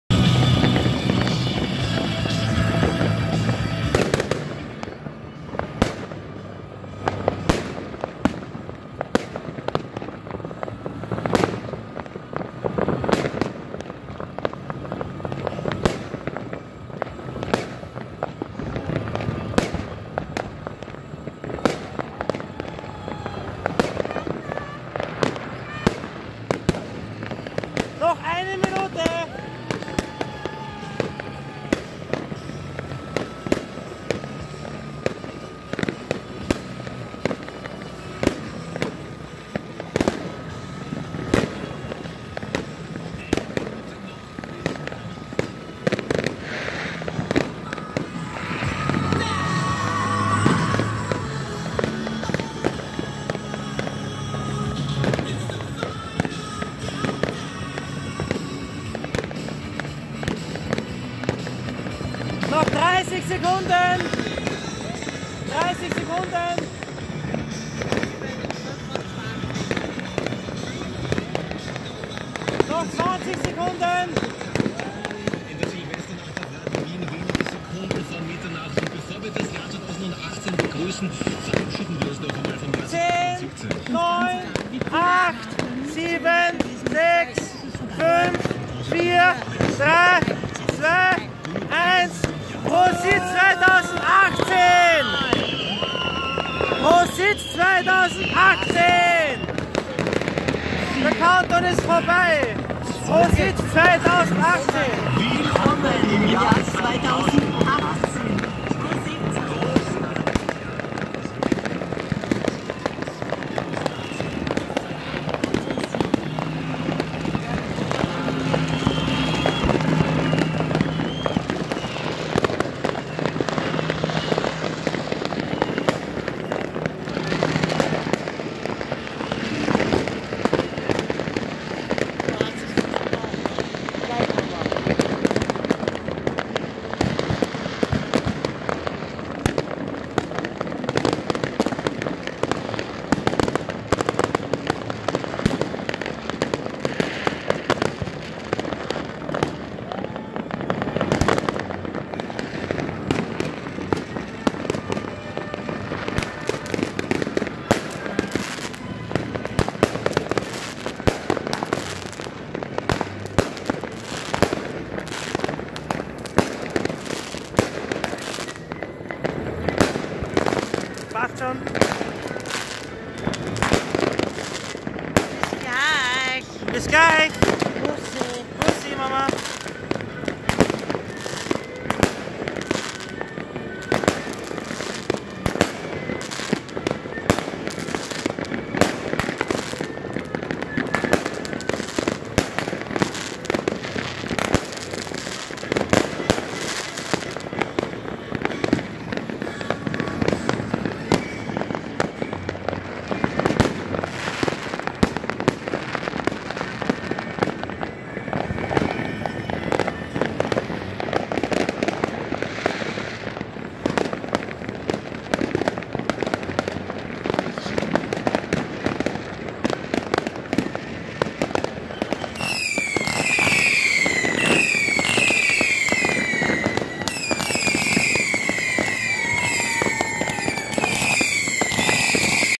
Jahreswechsel 2017 - 2018 in Wien-Breitenlee
Das war mein Jahreswechsel ohne eigene Raketen und Böller, sondern mit Infomonitor mit Countdowntimer, Musik von Radio Wien und meinem Scheppach SG-1200 Invertergenerator